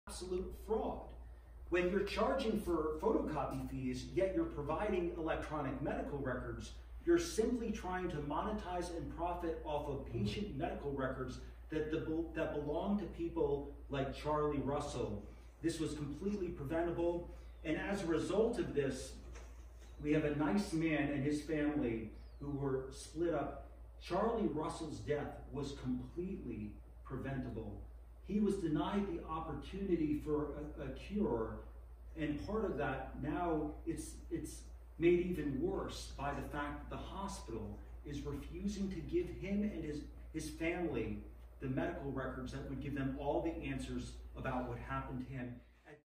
HealthAlliance Lawsuit
Attorney John H. Fisher of Kingston, NY talks about his client Sherry Russell’s lawsuit against HealthAlliance.